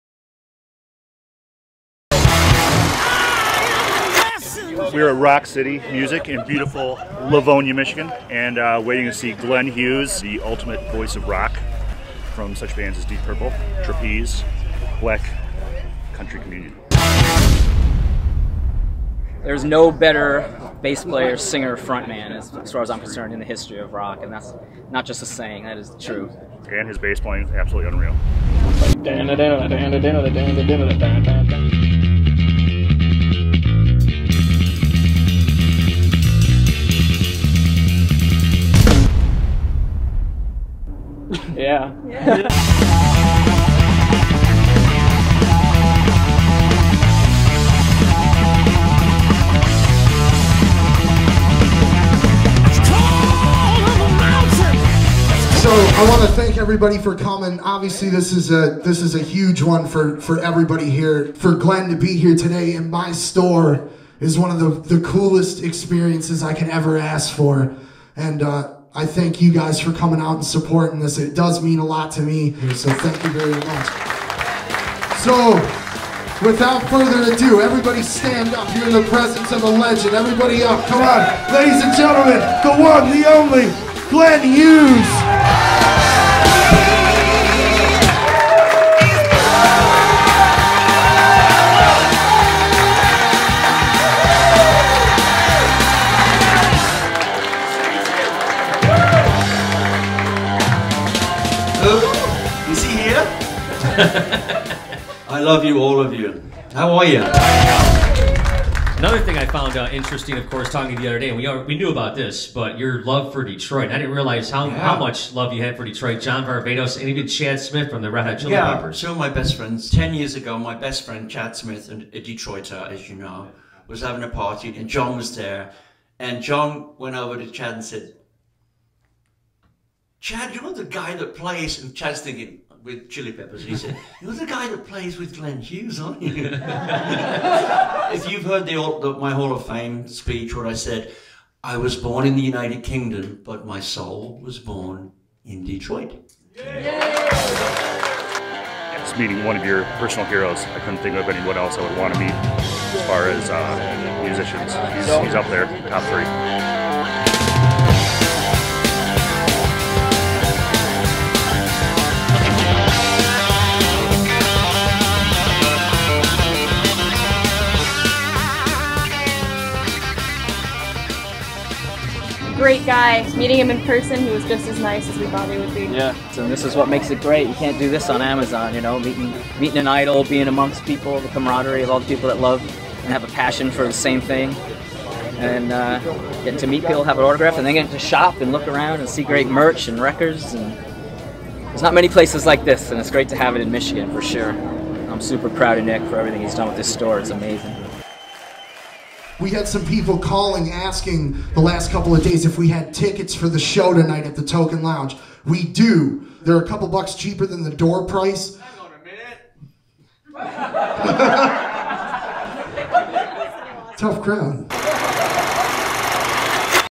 Glenn Hughes Exclusive In Store / Meet & Greet @ Rock City Music Company 9-15-18
Orange Amplification Presents: The Voice of Rock Glenn Hughes (Deep Purple, Trapeze, Black Sabbath, & Black Country Communion). Glenn made an appearance at Rock City Music Company on September 15th 2018. The appearance consisted of a question and answer session hosted by Meltdown of 101 WRIF. Right after Glenn signed autographs and took photos with his fans.

 A Chris Honoway Film